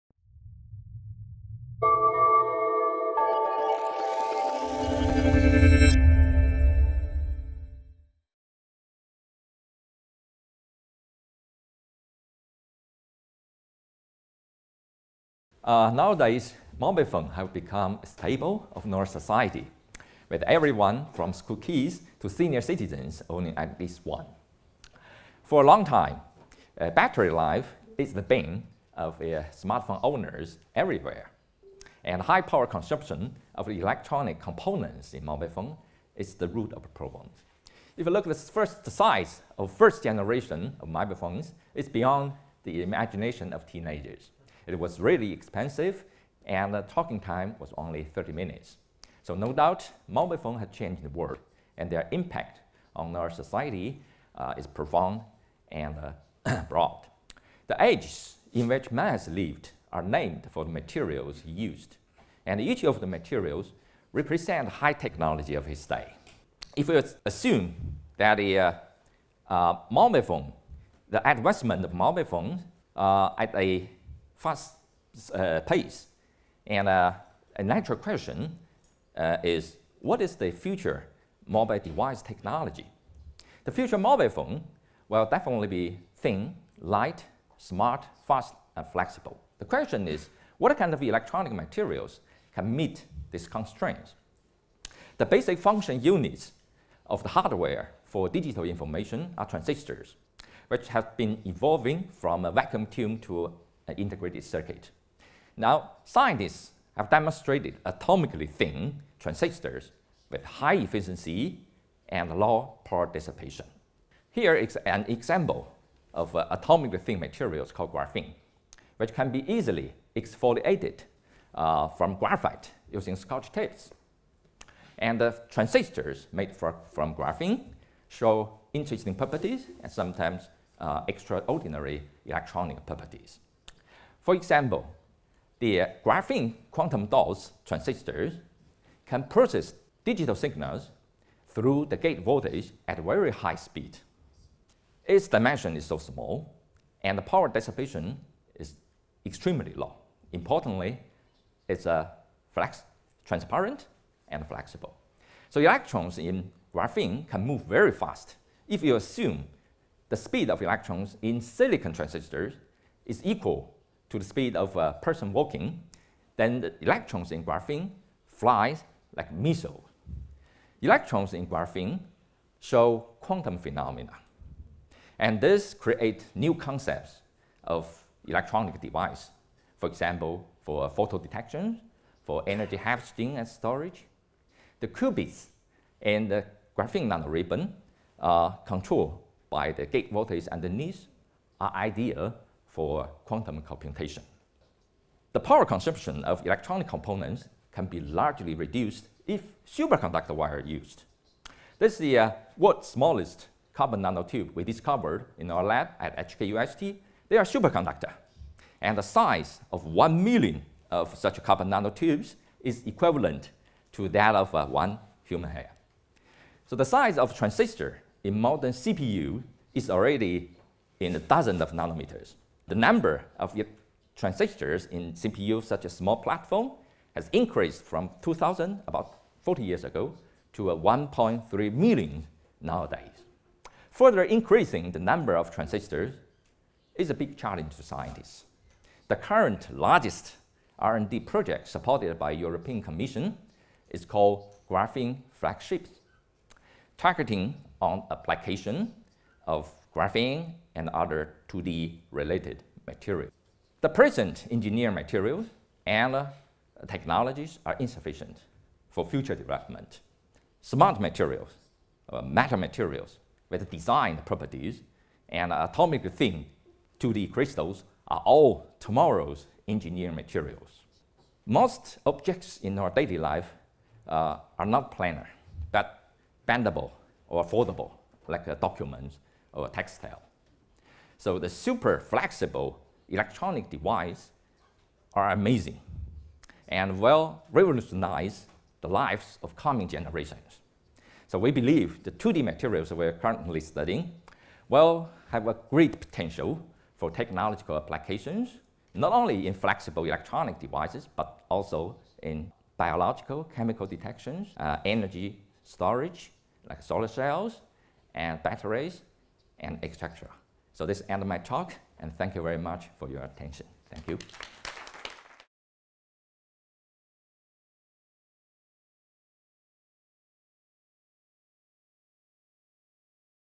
Paper-thin smart devices | Wang Ning
Ning Wang discusses the quantum possibilities of future mobile devices. The Professor of Physics, from Hong Kong University of Science and Technology, says nanomaterials like graphene will make tomorrow’s mobile phones superlight and flexible.